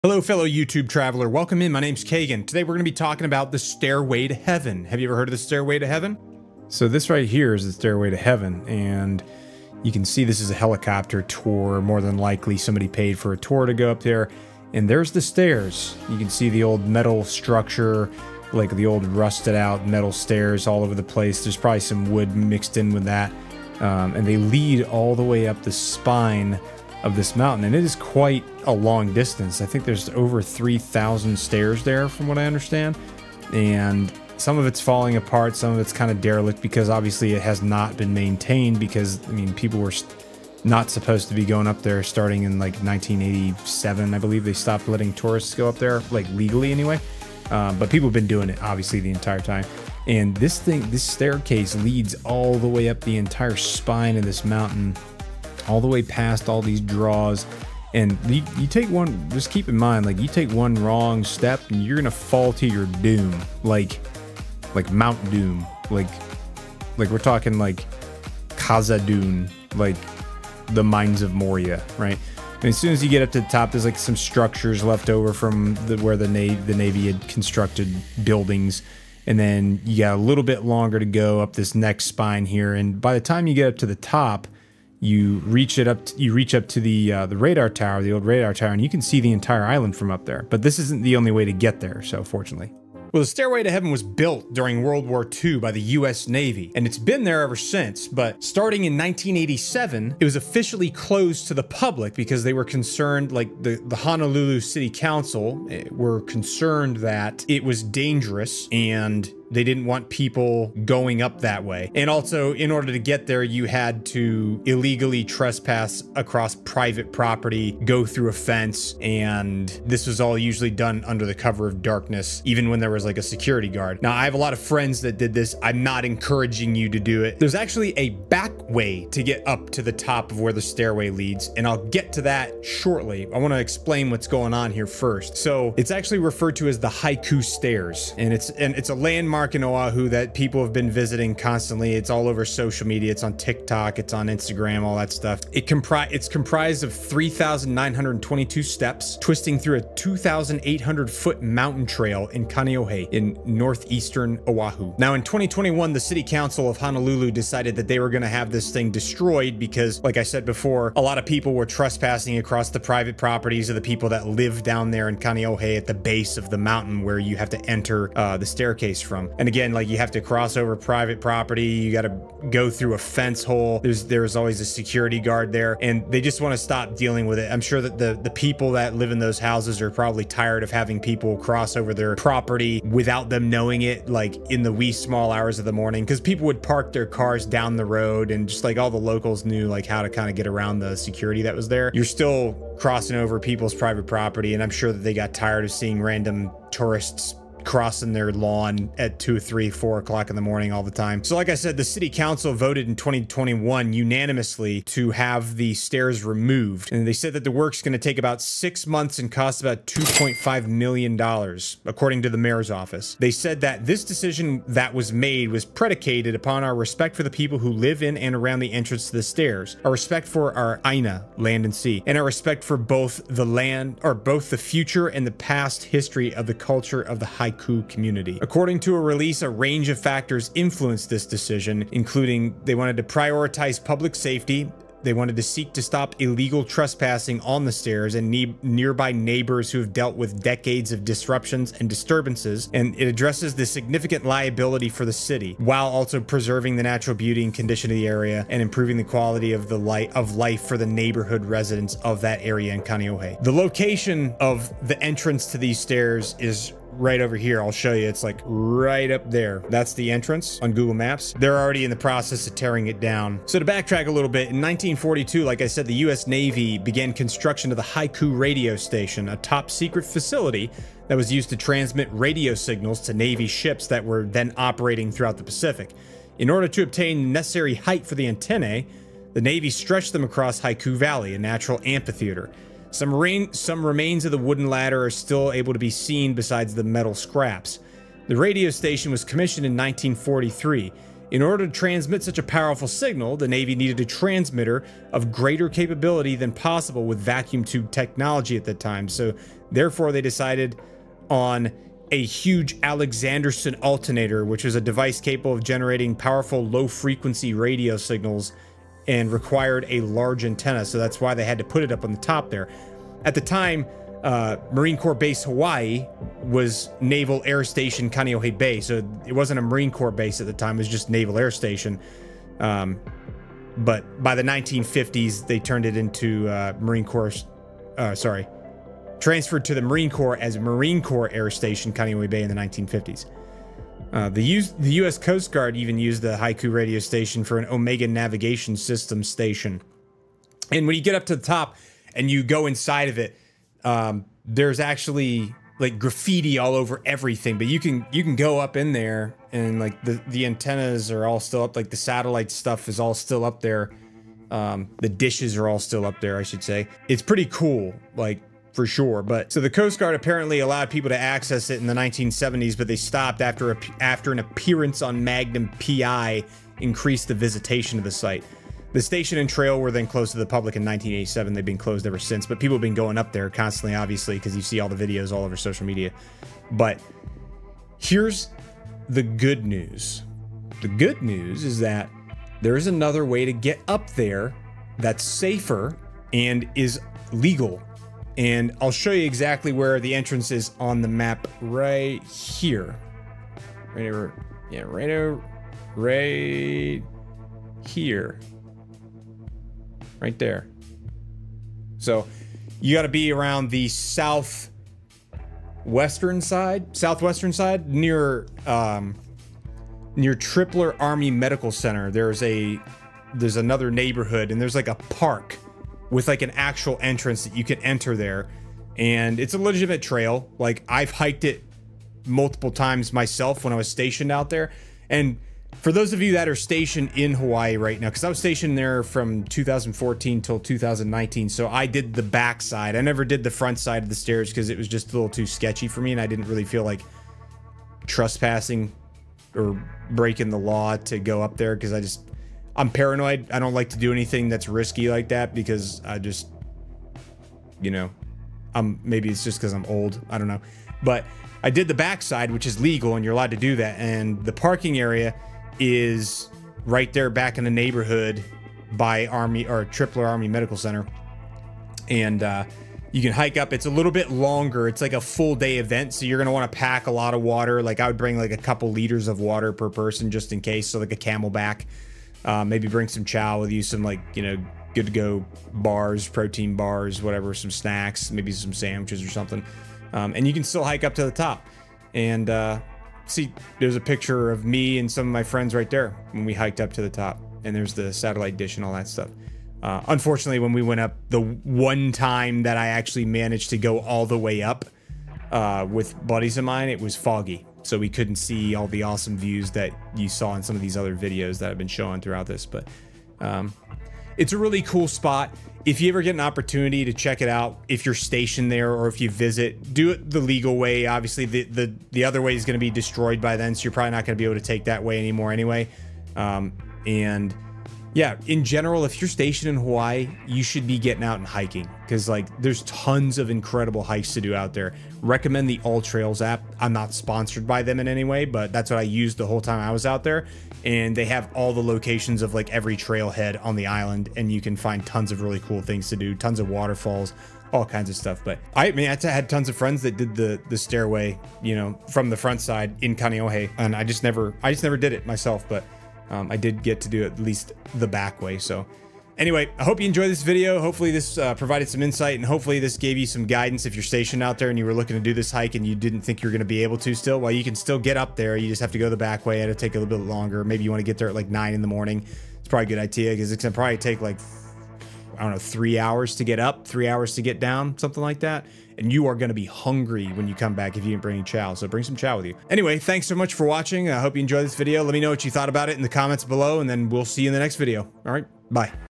They're Destroying the Stairway to Heaven in Hawaii
If you live in Hawaii or you're planning on visiting, they're destroying the stairway to heaven. But there is still a way to get up to the top.